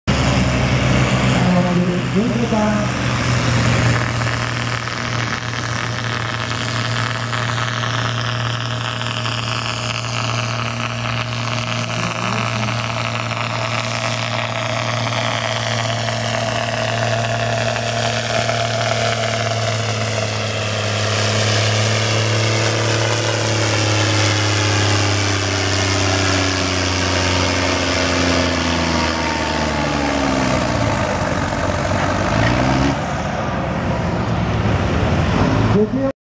2017 05 25 TT Hoogeloon Johan Schelle 6,5ton 1e trek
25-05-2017 Trekkertrek Hoogeloon - Johan Schelle  CaseIH 1255xl - 6,5ton standaard 1e trek